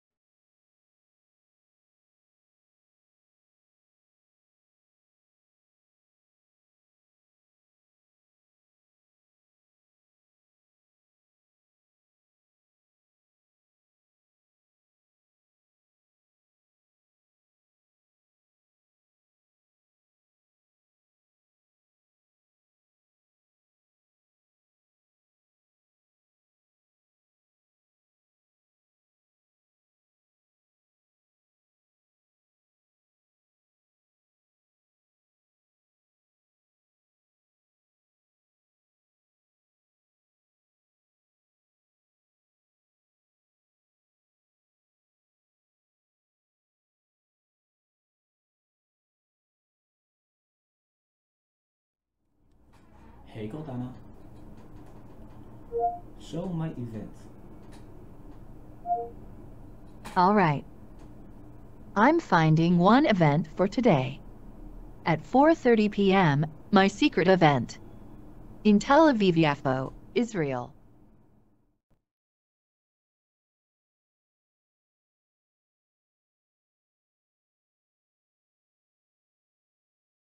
Cortana Calendar Skill Vulnerability (Windows 10 Lock Screen Bypass)
Security vulnerability in Cortana - the flaw allows to view all calendar events on the Lock screen.

The vulnerability was found by Yuval Ron and Amichai Shulman as part of academic research at the Technion - Israel Institue of Technology, under the supervision of Prof. Eli Biham.

חולשת אבטחה בקורטנה העוזרת הקולית של מייקרוסופט. החולשה דווחה ע״י יובל רון, עמיחי שולמן ואלי ביהם מהטכניון - מכון טכנולוגי לישראל.